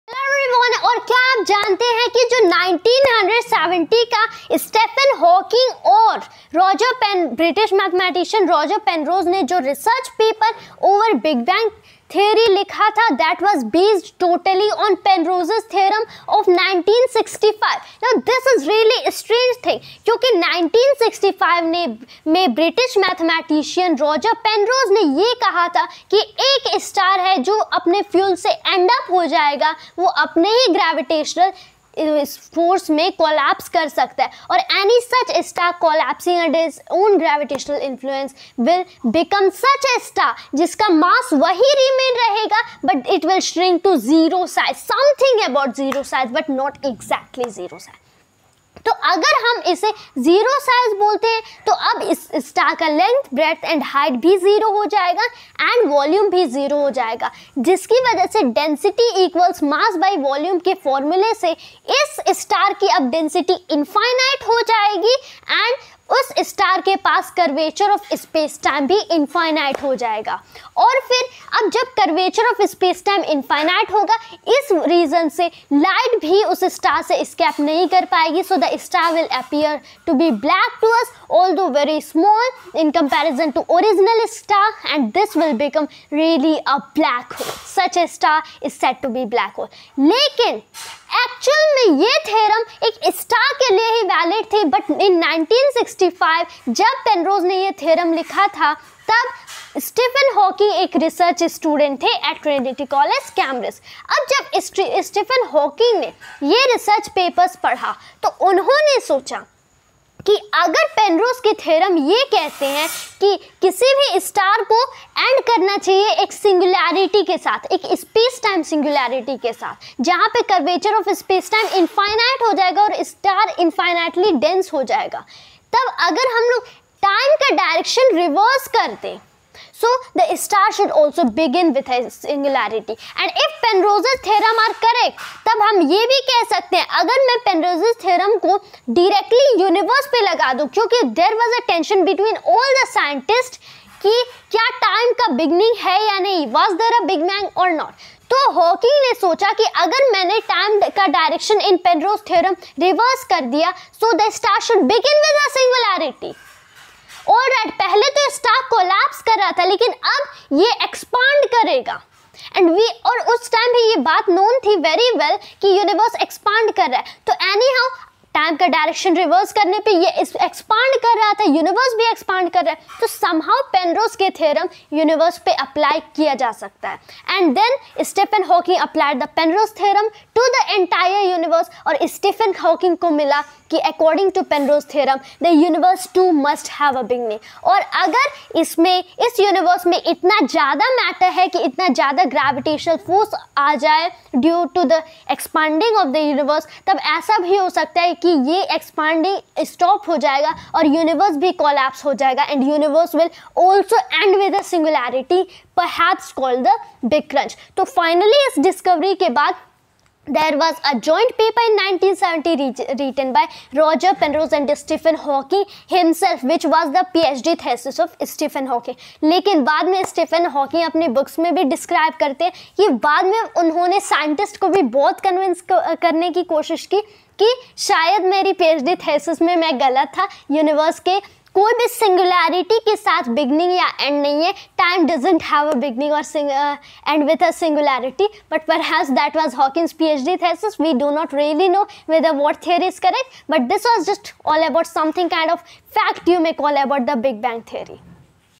Fact about 1970 Hawking/Penrose Research Paper about Big Bang
Do yo know that the research paper written by Stephen Hawking and Roger Penrose in 1970 about big bang was based on Penrose's Theorem?